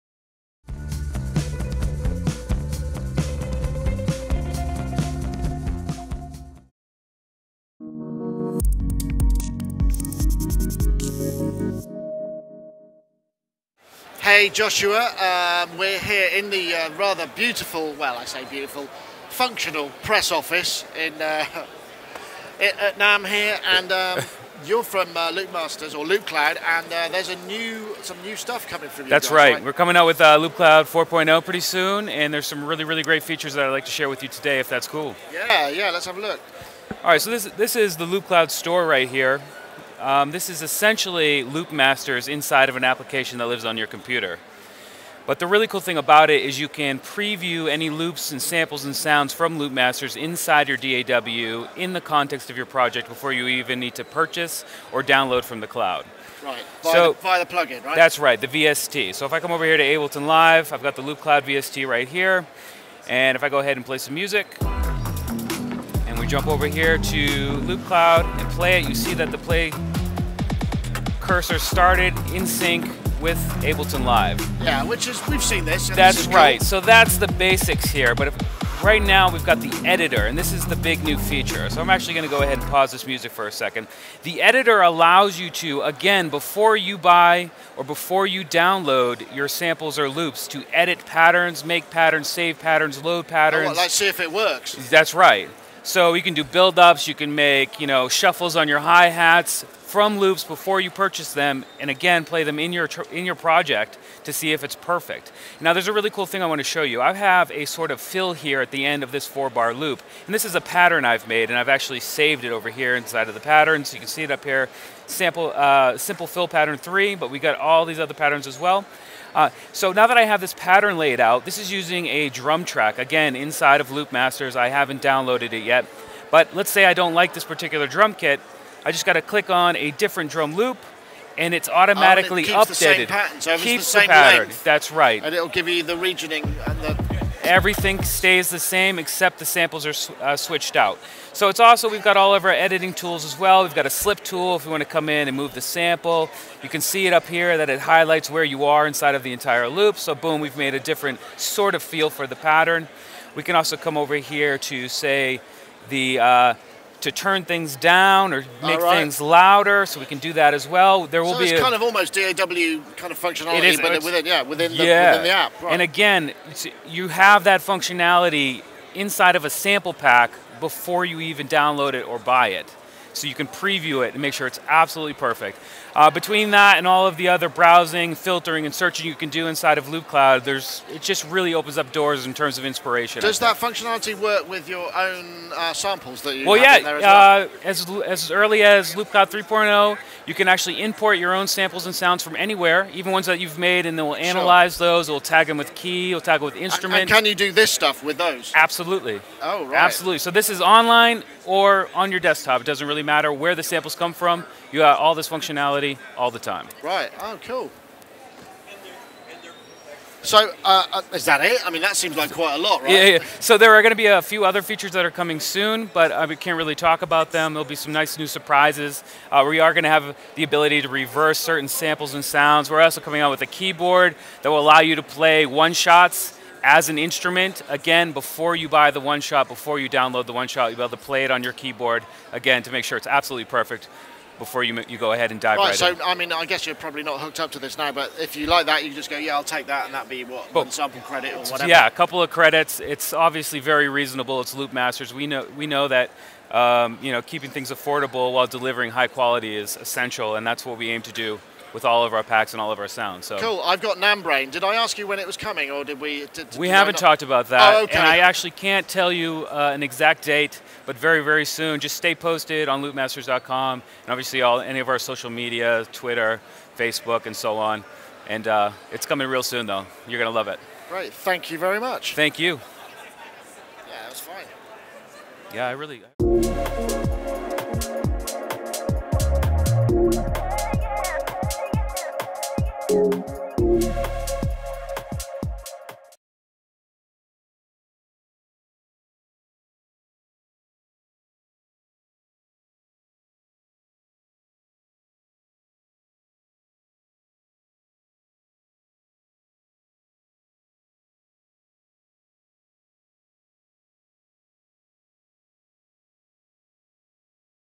NAMM 2019 Loop Cloud 4 Preview
Loopcloud were previewing their upcoming interface for version 4 of their free sample management system. With new audio editing  features which also apply to user samples as well as as yet un purchesed.